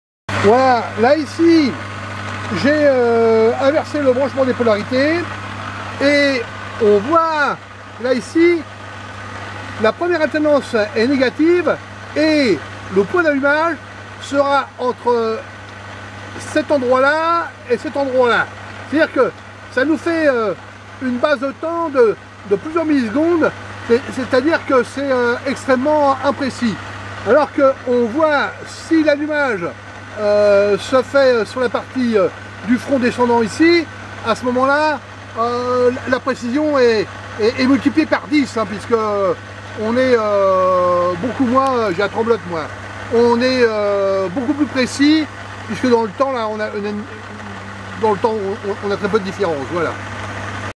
Signal délivré par un allumeur pour déclencher l'étincelle à la bougie. Ici le signal reçu est inversé et bien que cela fonctionne tout de même nous aurons malheureusement un point d'allumage imprécis